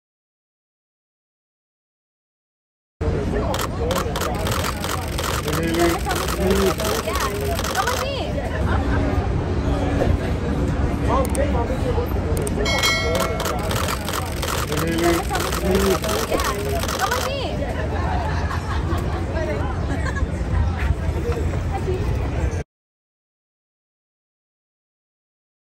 Hailey Bieber leaving Bar Pitti after have lunch with friends.
Hailey Bieber leaving Bar Pitti after have lunch with friends in SoHo, New York City